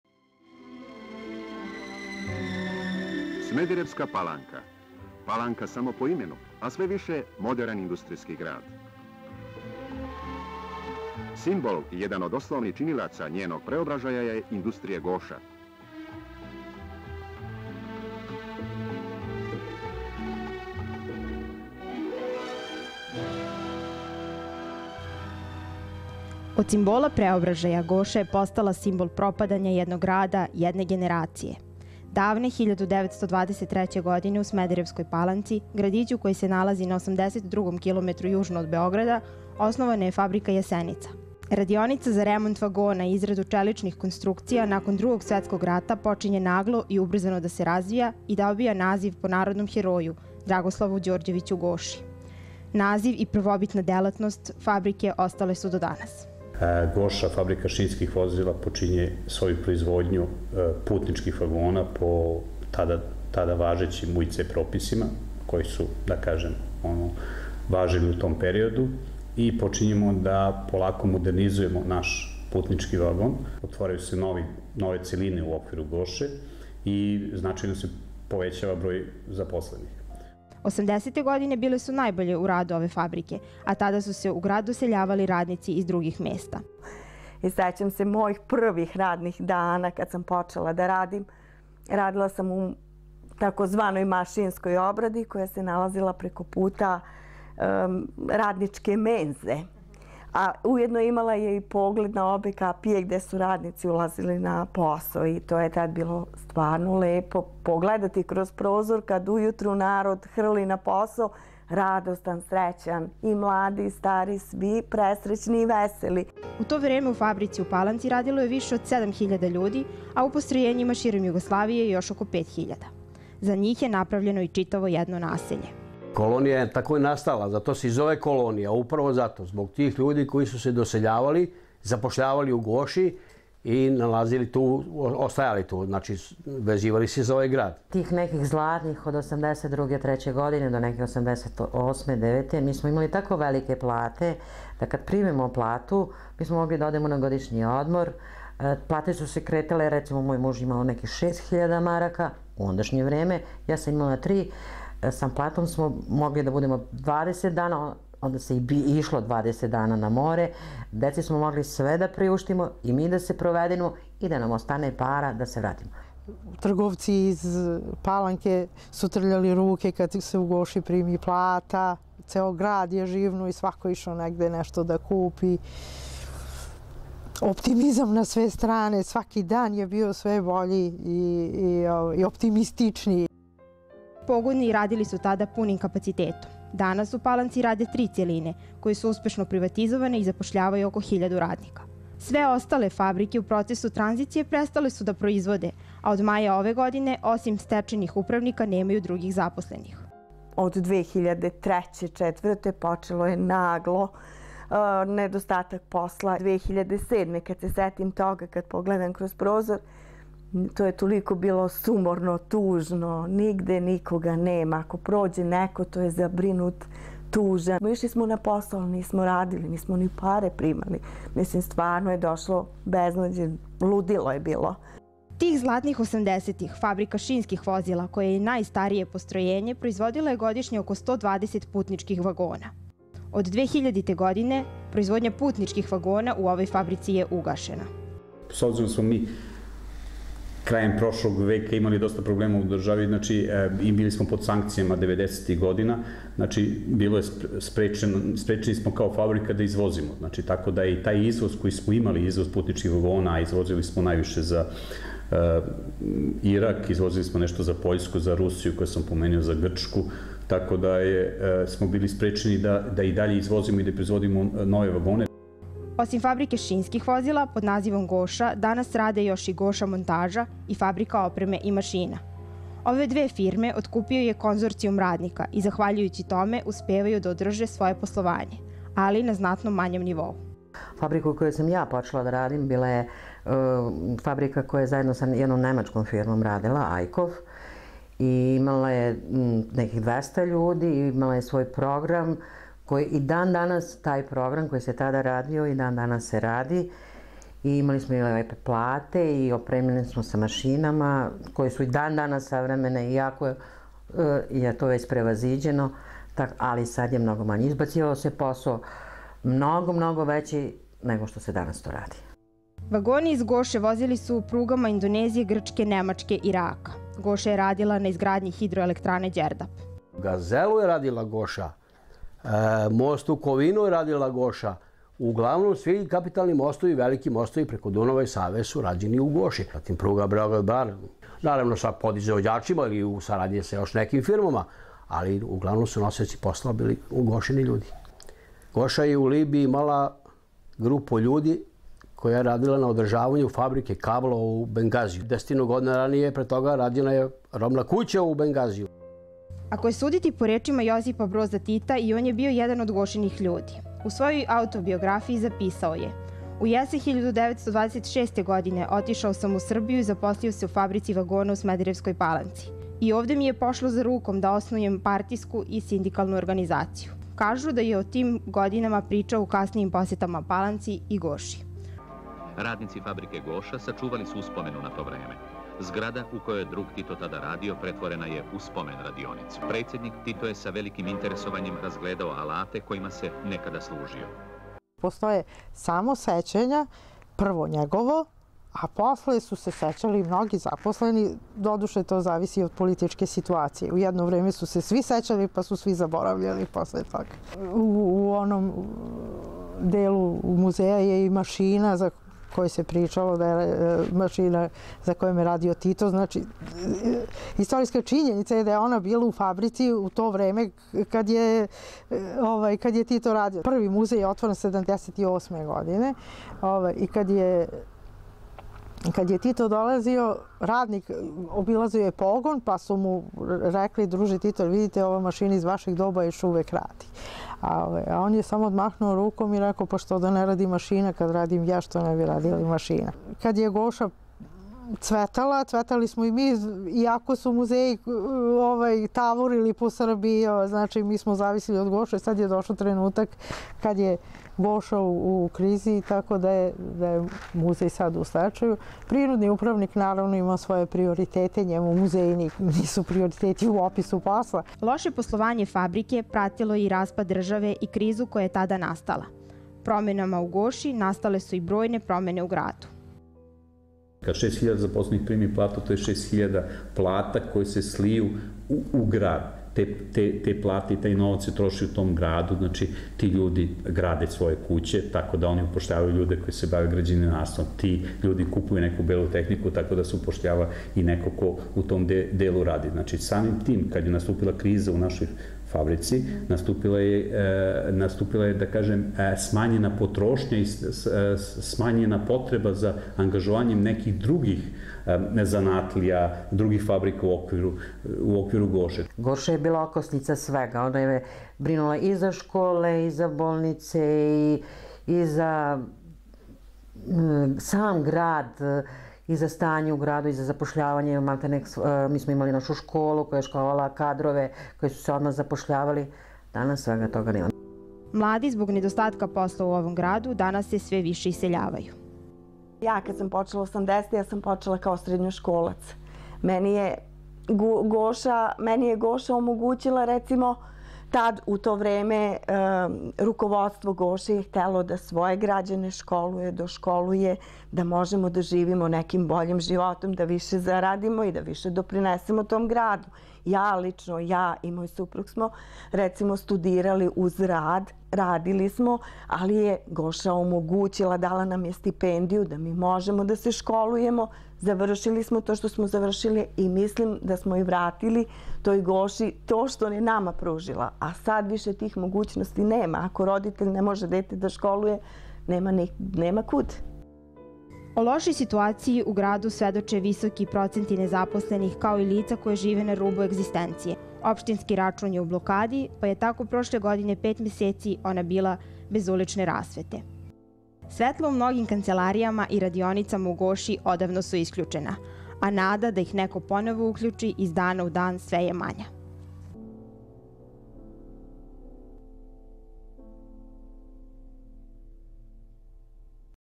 Goša, nekad i sad
U Smederevskoj Palanci 1923. godine osnovana je fabrika za remot vagona i izradu čeličnih konstrukcija "Jasenica" a.d. Fabrici je nakon drugog svetskog rata promenjeno ime i proširena delatnost. Tokom sedamdesetih i osamdesetih u njoj je radilo i do 12.000 ljudi, a bila je značajna za čitavu SFRJ.

"Goša" je gradila širom zemlje i sveta. Neke od najznačajnijih konstrukcija ove fabrike u zemlji svakako su most Gazela, Brankov most, HE Đerdap, železnički mostovi duž pruge Beograd-Bar... Radnici ove fabrike gradili su po Libiji, Bengaziju, Iraku, Alžiru... Vagoni iz Goše i dalje voze evropskim i svetskim prugama, a Beogradom tramvaji nastali ili remontovani u ovoj fabrici.

Zanimljivo je da je i Tito bio jedan od radnika ove fabrike. On je svoj bravarski zanat pekao 1926. i 1927. u Palanci.

Goša danas zapošljava nešto više od 1.000 ljudi, većina fabrika ove holding korporacije zatvorena je, a proizvodnja ugašena. Ovaj master rad pokušaj je da se dokumentuje ono što je Goša bila nekada i onoga što je danas.